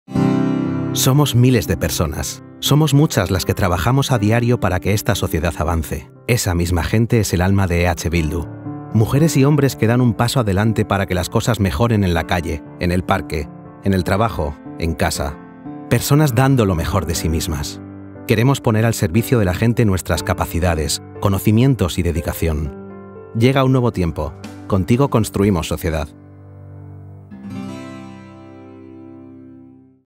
¡Llega un nuevo tiempo! Contigo construimos sociedad
Somos muchas las que trabajamos a diario para que esta sociedad avance. Esa gente es el alma de EH Bildu. Queremos poner al servicio de la gente nuestras capacidades, conocimientos y dedicación.